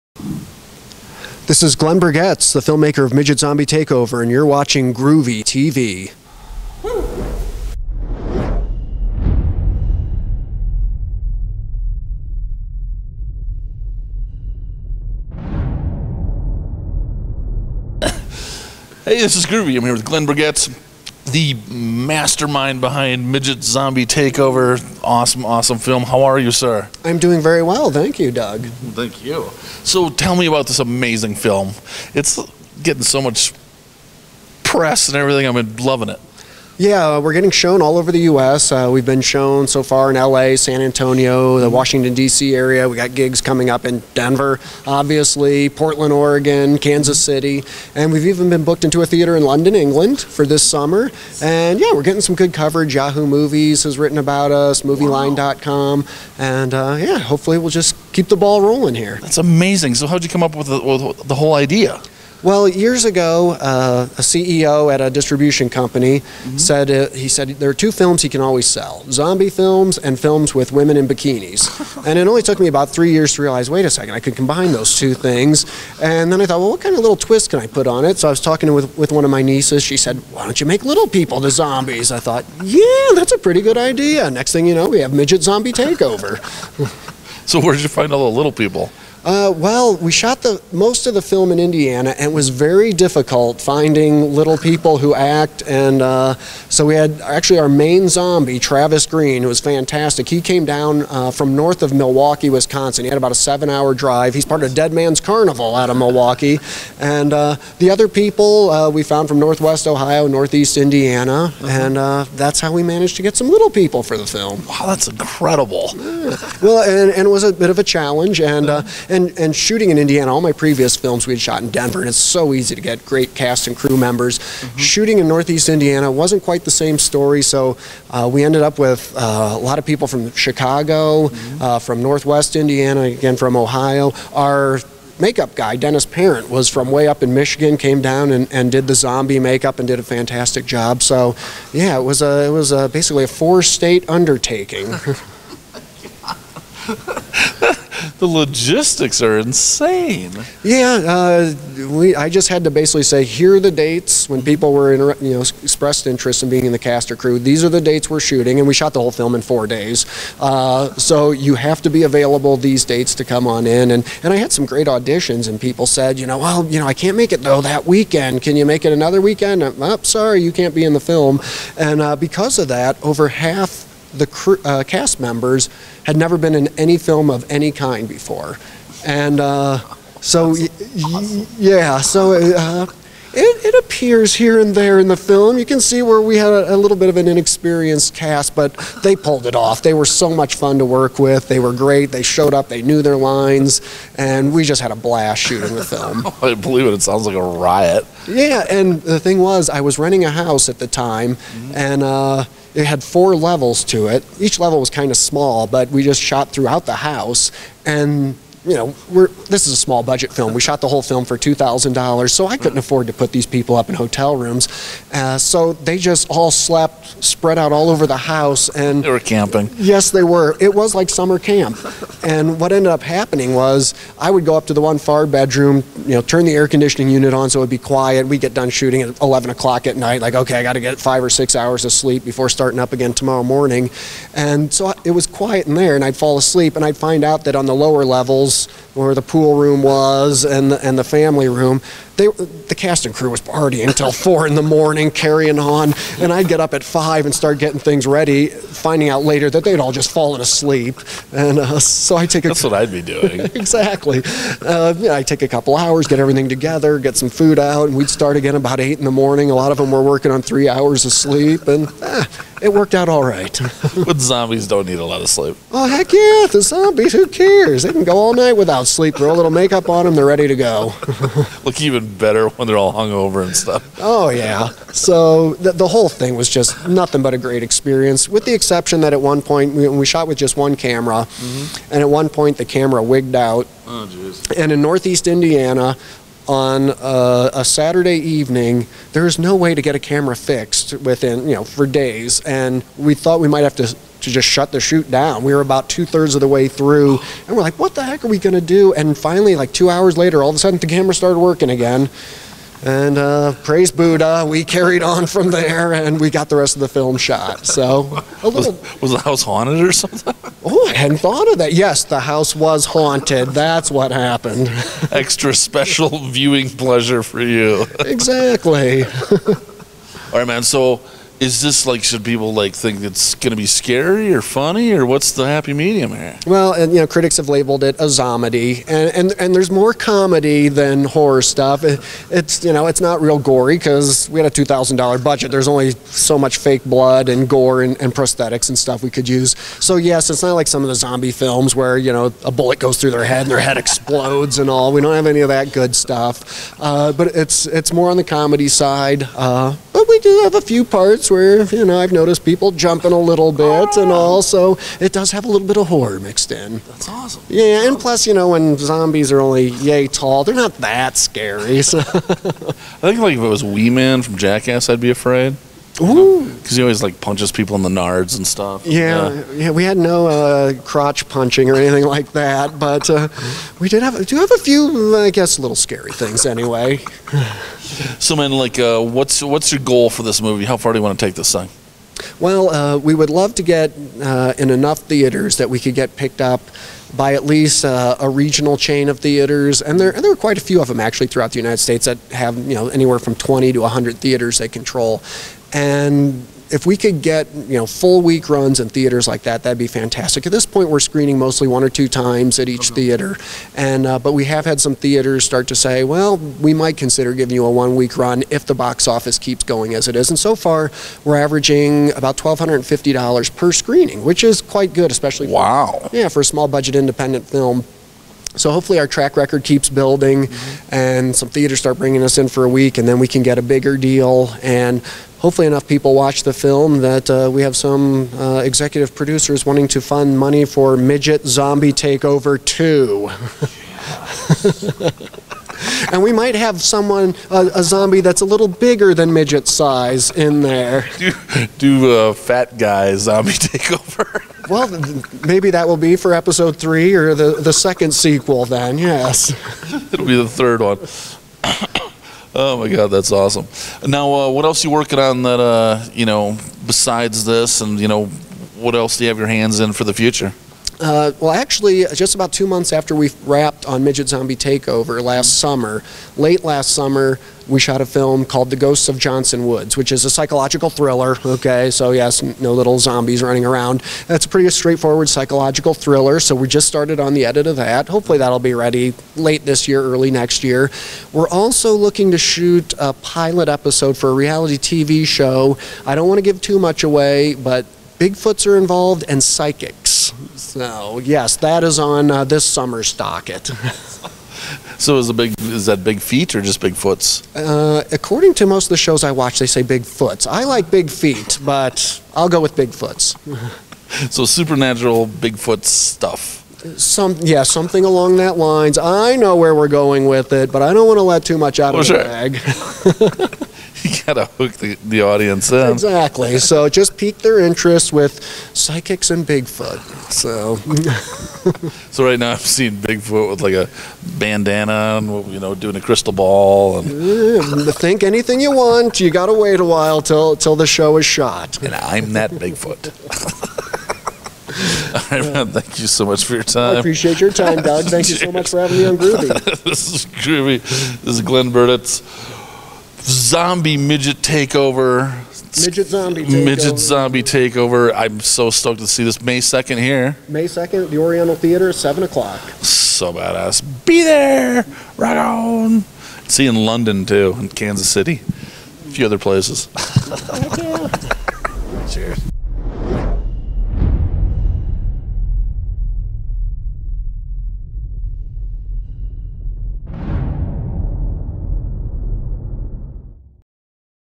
Midget Zombie Takeover Creator Glenn Berggoetz Interview
Groovey.TV interviews Midget Zombie Takeover Creator Glenn Berggoetz at The Oriental Theatre.